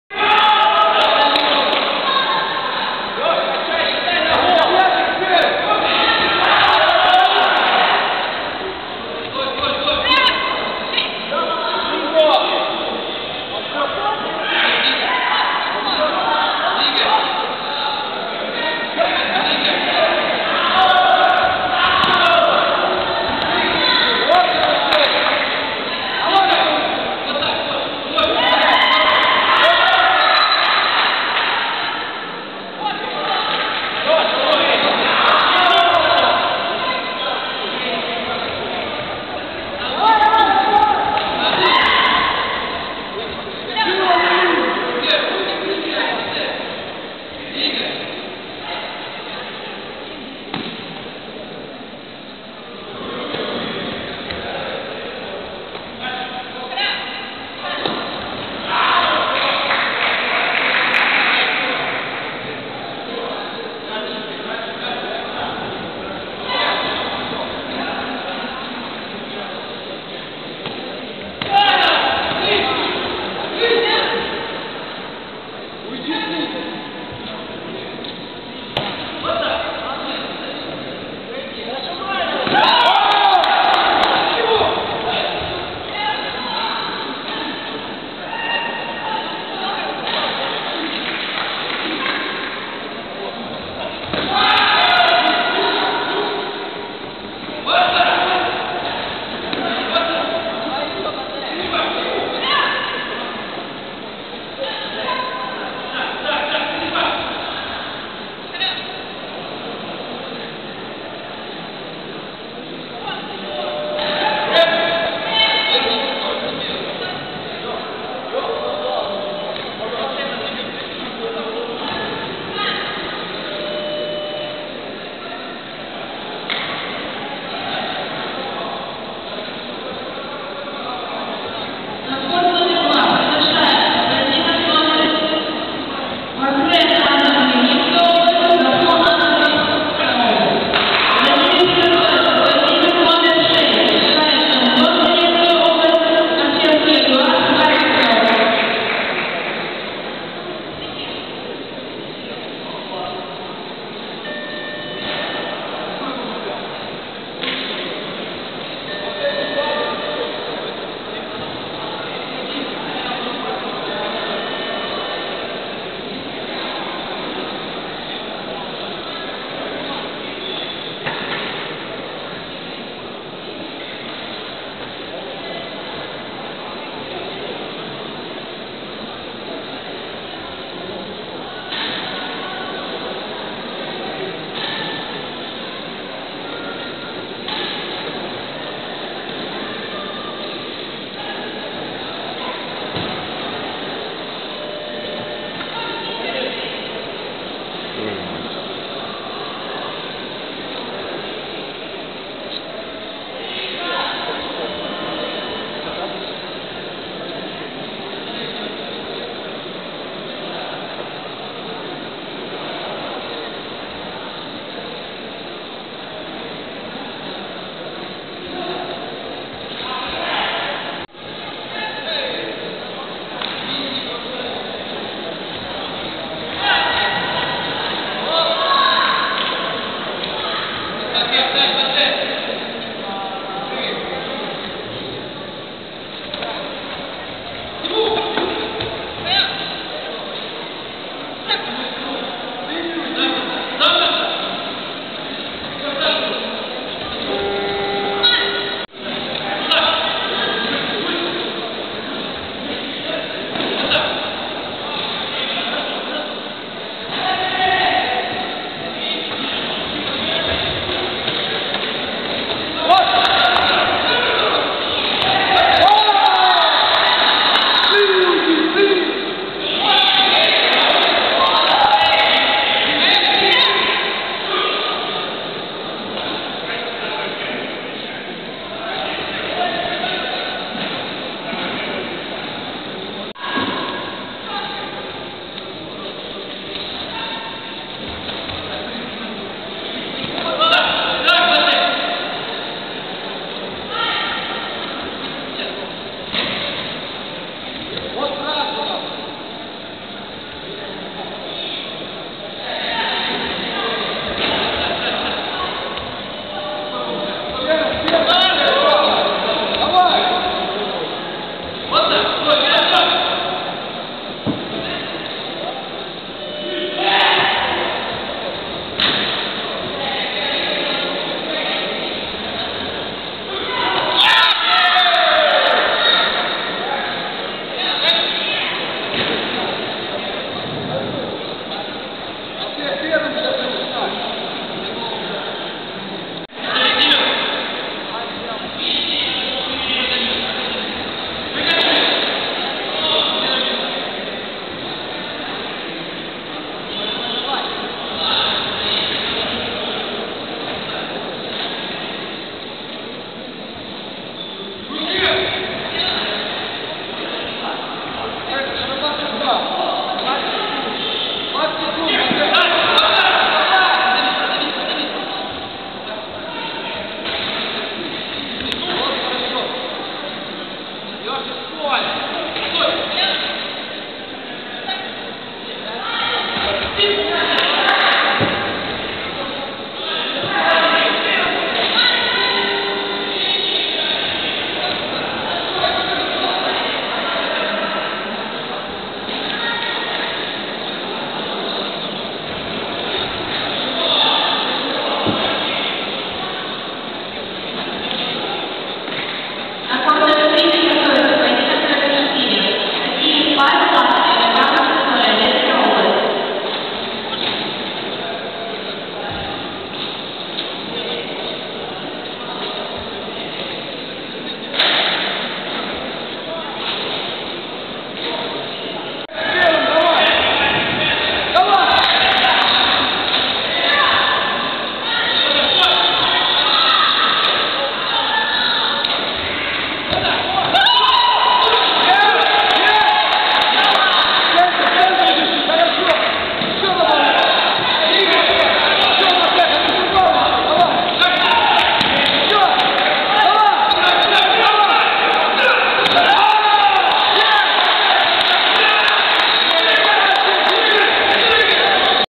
Ukraine Open 2011: Жеребчевский Харьков
Жеребчевский Алексей в спаринге против Киевлянина на Открытом кубке Украины 2011 в Киеве